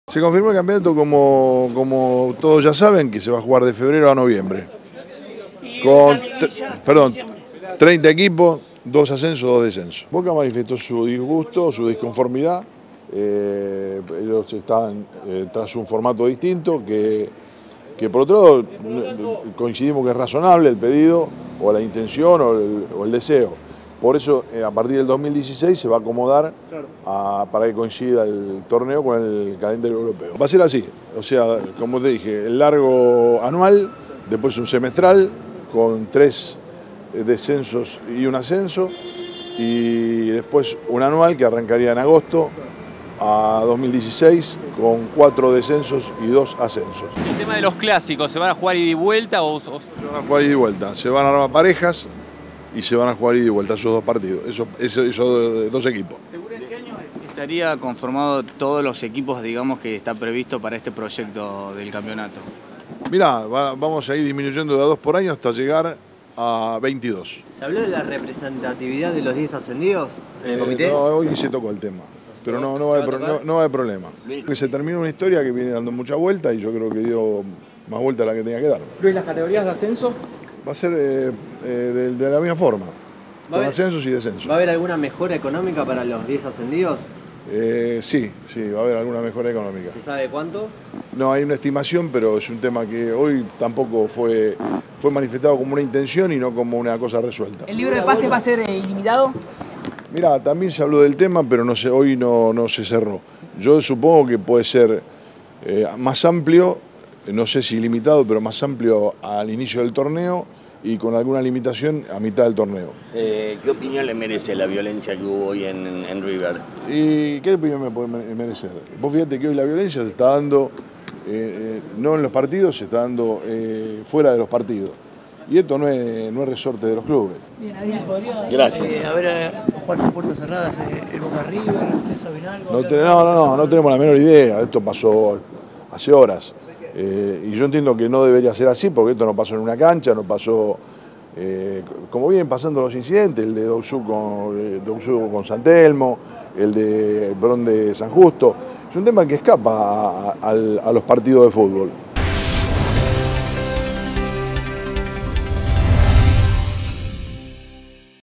El torneo 2015 será de febrero a noviembre con dos ascensos y dos descensos
El Comité Ejecutivo de la Asociación del Fútbol Argentino (AFA) aprobó el campeonato de Primera División 2015, que contará con la participación de 30 equipos, a celebrase entre febrero y noviembre, que tendrá un campeón y dos descensos.

El nuevo formato del torneo 2015 propuesto el pasado 4 de junio por el fallecido ex presidente de la AFA, Julio Humberto Grondona, fue aprobado por votación mayoritaria, ya que Boca Juniors estuvo en desacuerdo.